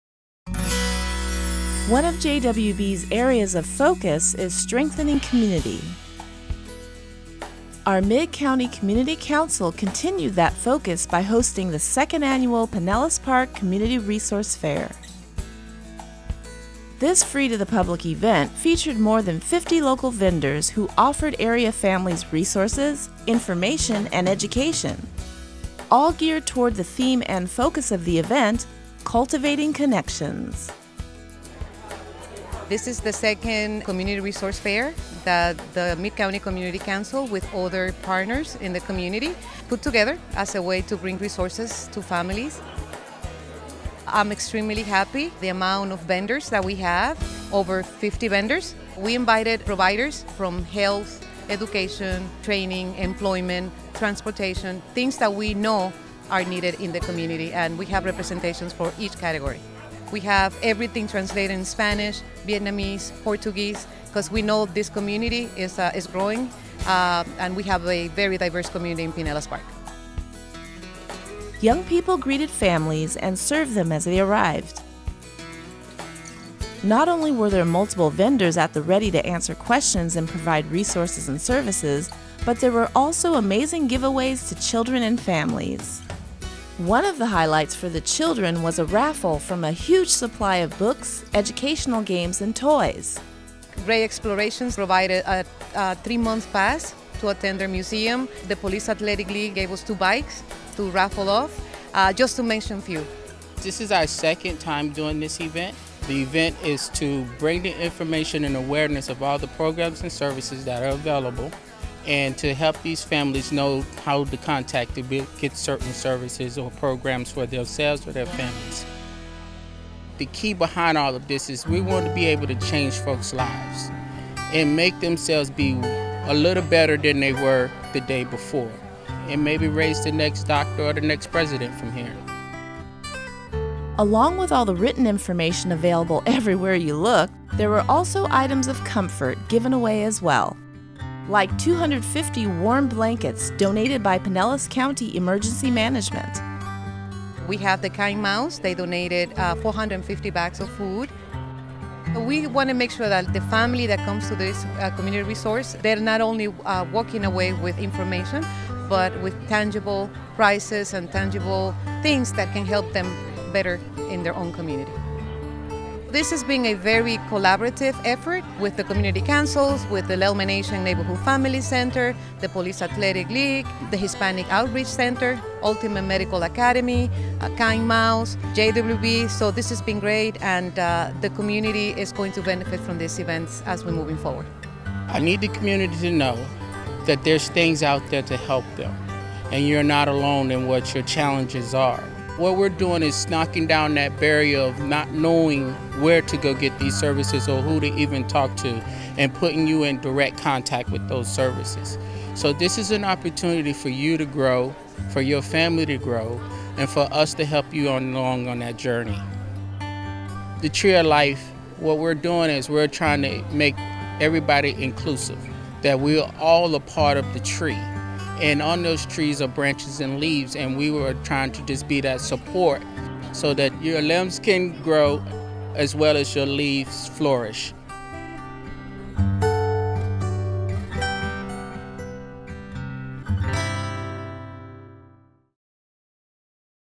Pinellas Park Community Resource Fair
Members of JWB’s Mid-County Community Council hosted their 2nd Annual Community Resource Fair for Pinellas County families at the Pinellas Park Performing Arts Center.

Bringing together more than 50 social service and healthcare vendors, this well-attended event featured something for everyone in terms of information, resource, and education.

to learn more about JWB's Community Councils.